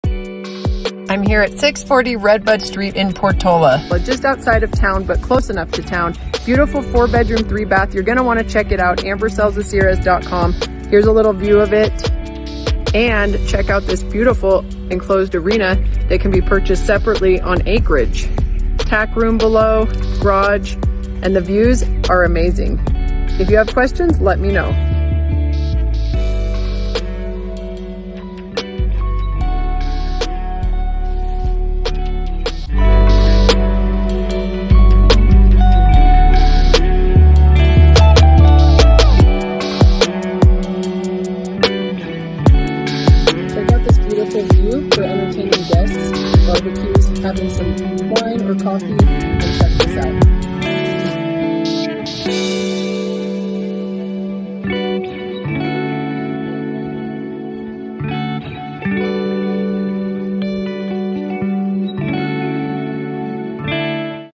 New Listing in Portola, CA by Amber Donnelly | 📍640 Redbud St. Portola, CA
New Listing in Portola, CA by Amber Donnelly

📍640 Redbud St. Portola, CA
$525,000

This is a 2 Story house 🏡 with
🛌 4 Bedroom
🛀 3 Baths
2 Detached Garage

If you're considering selling or buying, I'm here to HELP!